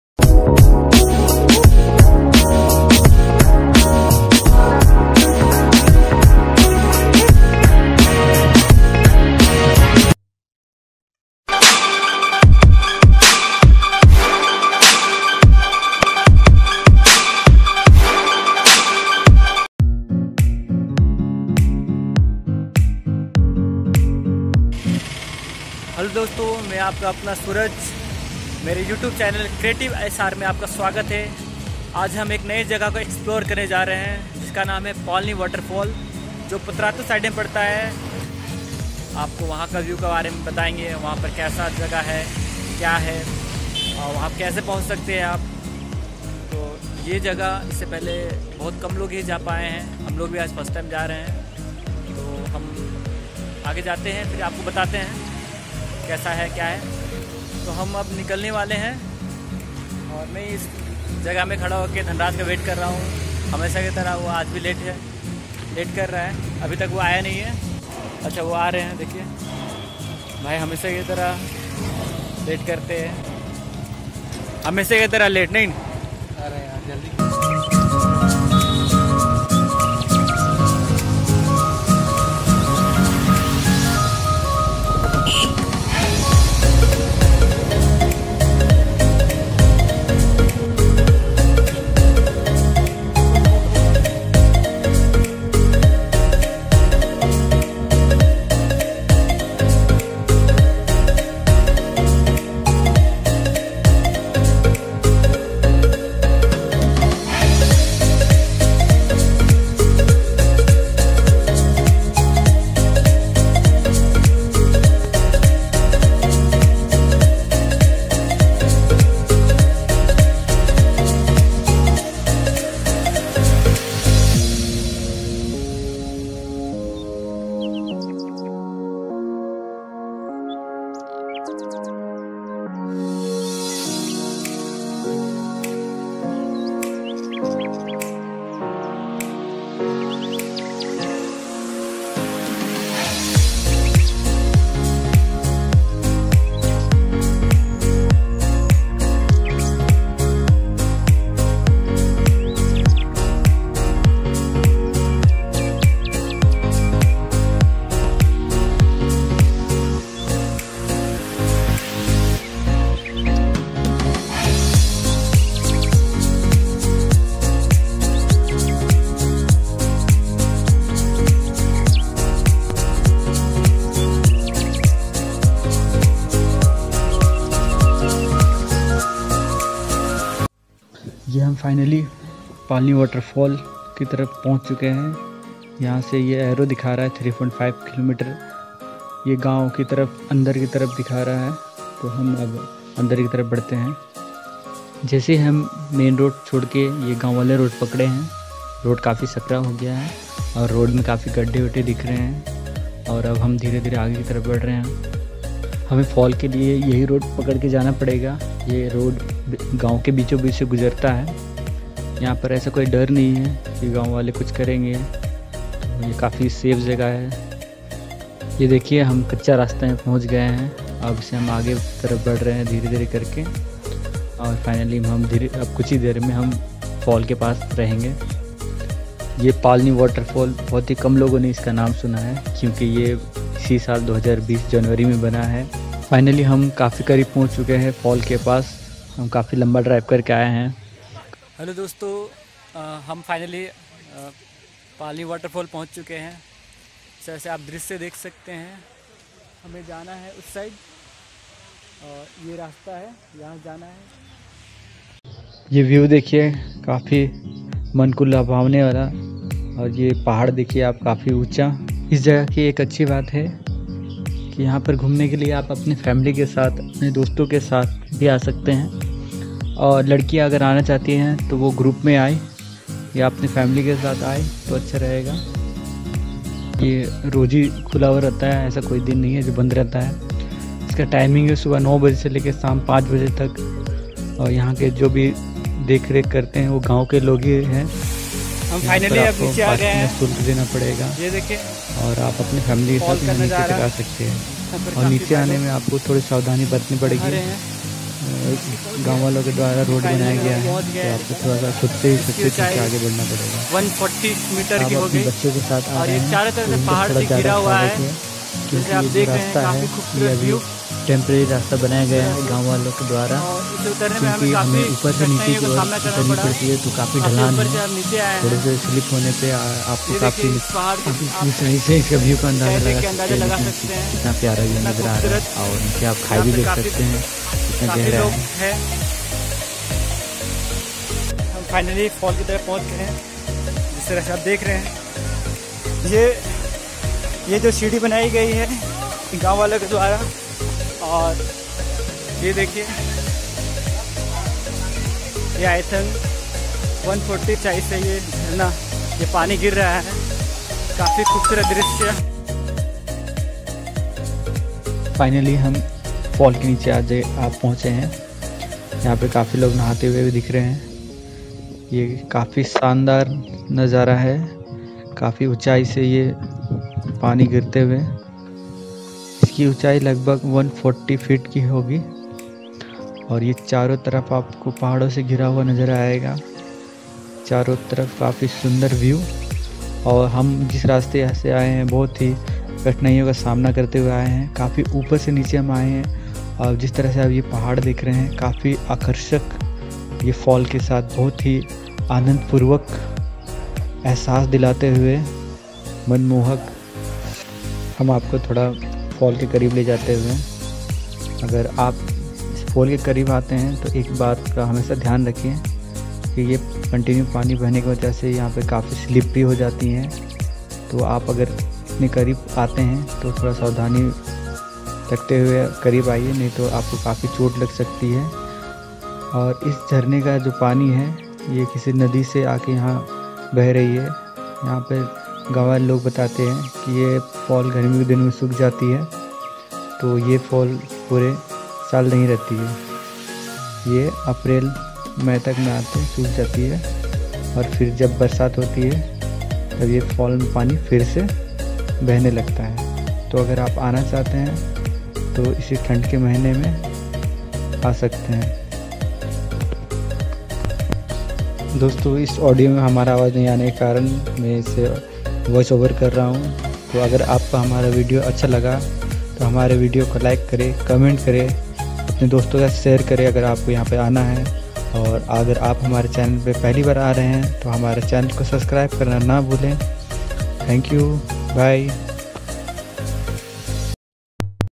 Palani Water Fall ( पालनी जलप्रपात ), ( Hidden Piace in Patratu ),Ramgarh, Jharkhand
पालनी जलप्रपात रांची से 50 km की दूरी में है यह जगह जनवरी 2020 से सुरू हुई है , इस लिए बहुत से लोगो को इस जगह के बारे में पता नही है । इस जलप्रपात की ऊँचाई 140 फिट की है ,इसके चारो तरफ से पहाड़ से घिरा हुआ है , आप आने कार ,bick से जा सकते है ,इस जलप्रपात के पास पहुचने के लिए  कोई पक्का रास्ता नही बना है जो भी रास्ता बना है वो इस गांव के लोग के द्वारा बनाया गया है । इस झरने के पास जाने के लिए नीचे उतरना पड़ता है , नीचे उतरते वक्त थोड़ा सावधानी से उतरना पड़ता है qki अभी यह पक्का रास्ता नही बना है कभी भी हादसा हो सकता है ।